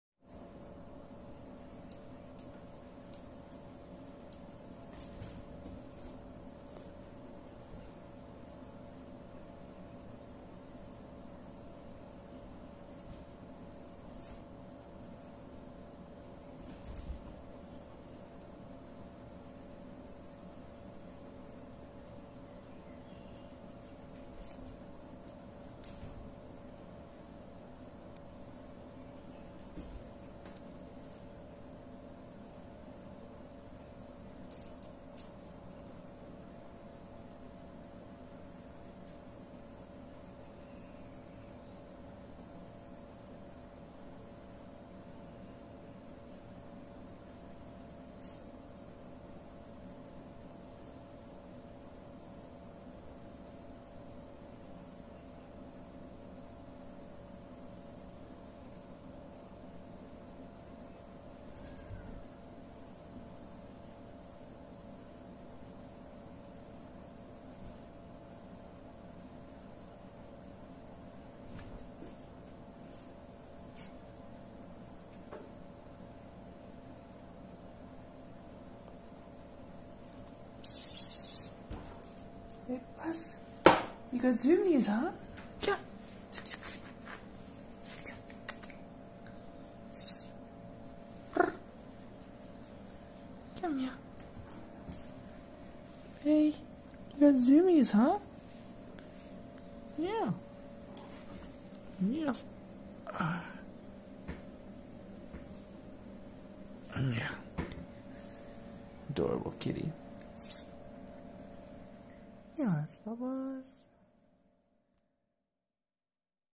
My cat has zoomies
My kitty with a case of the zoomies.

Sorry for the bad quality, it was dark and I didn't want to distract her from her zooms.